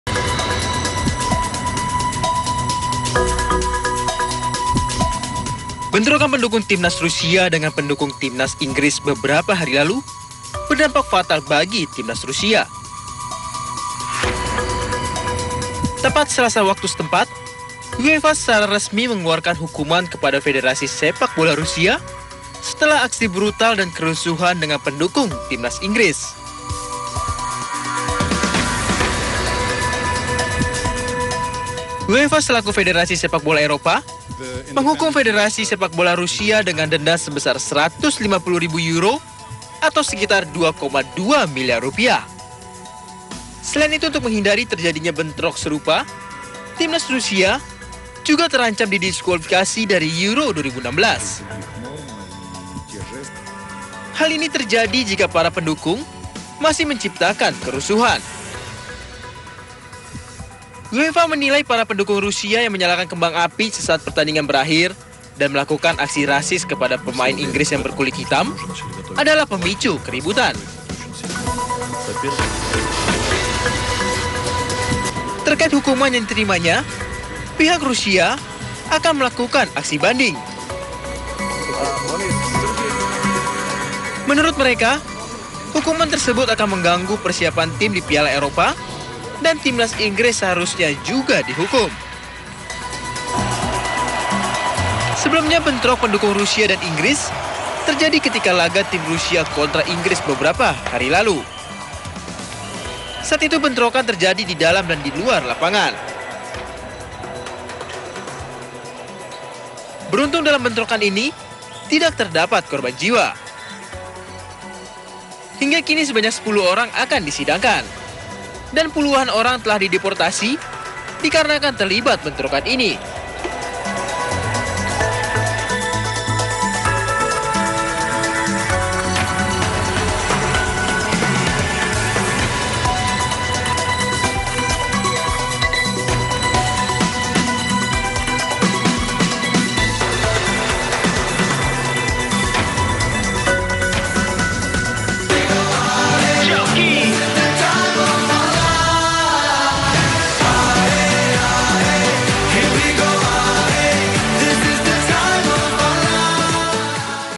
Suporter Inggris dan Rusia Bentrok Usai Laga
Laga Inggris vs Rusia di penyisihan Gurp B Piala Eropa 2016 diwarnai dengan aksi tawuran antar suporter kedua negara.fans Inggris diserang oleh suporter garis keras (ultras) Rusia di dalam Stade Velodrome setelah kedua tim bermain imbang 1-1.